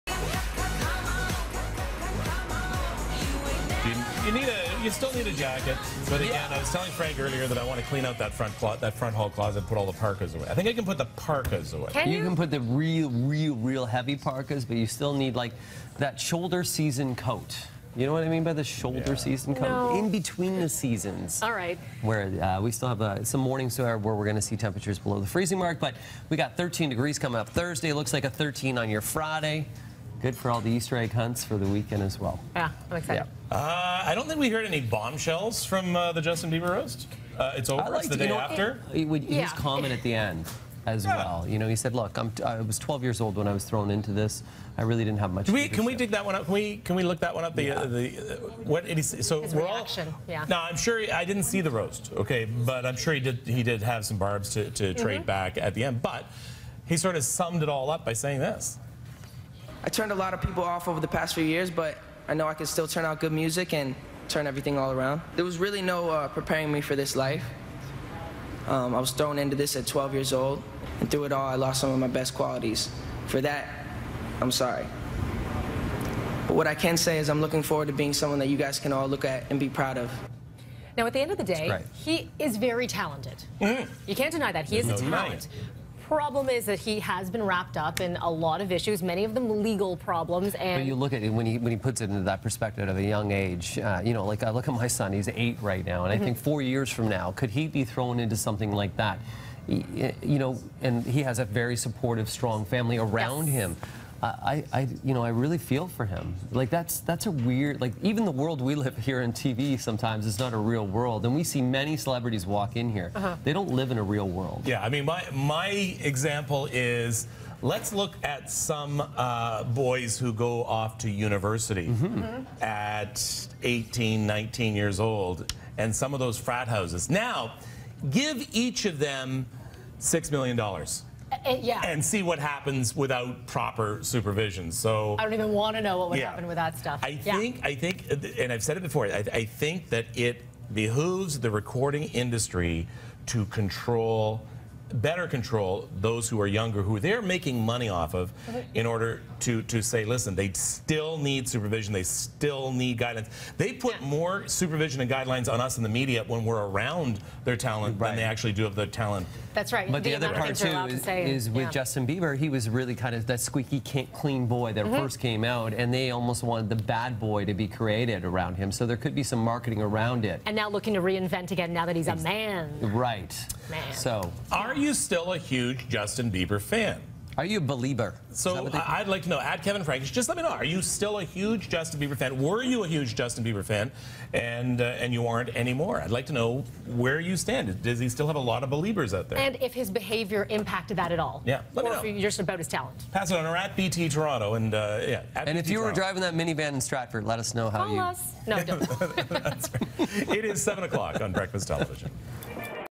Your Reaction: The Justin Bieber Roast
Justin Bieber ended his roast last night with an apology for his actions, are you still a 'Belieber'?